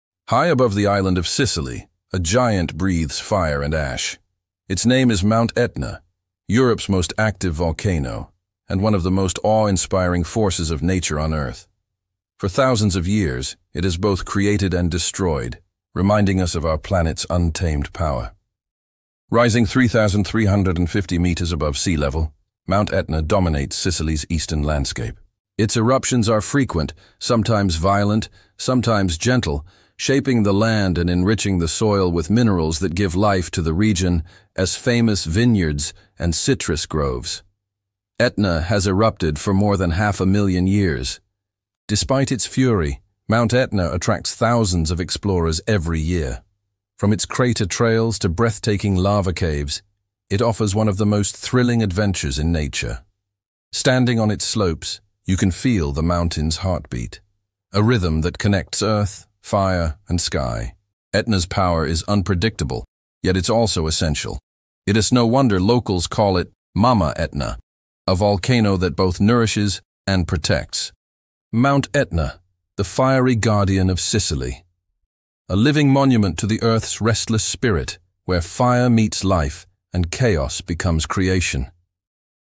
Mount Etna – Italy’s Fiery Giant Europe’s Most Active Volcano
Explore Mount Etna, the legendary volcano of Sicily, Italy — the most active volcano in Europe and one of the world’s most spectacular natural wonders. In this 5-minute documentary, witness the breathtaking power of lava flows, ash clouds, and eruptions that have shaped the island for thousands of years. Learn about its ancient myths, its vital role in the region’s ecosystem, and why Mount Etna continues to captivate scientists, locals, and travelers alike. From destruction to creation, discover the fiery force that defines the heart of Sicily.